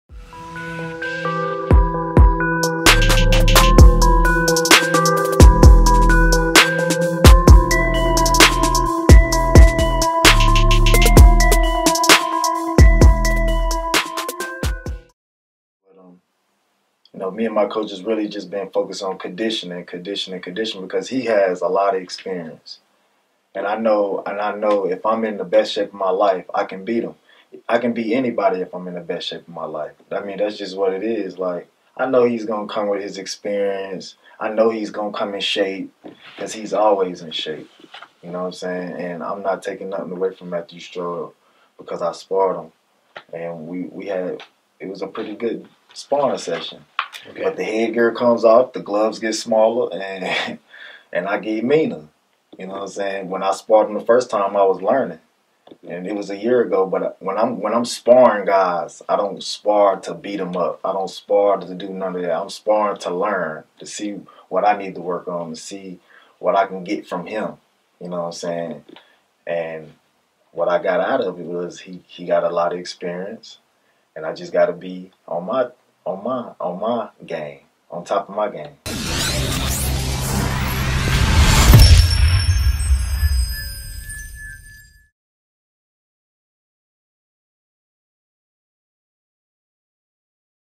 Keys to Win Joe "Action" Jackson
Joe "Action" Jackson tells the audience what it will take to beat his opponent on April 20th at the Meridian Convention Center in Greensboro, NC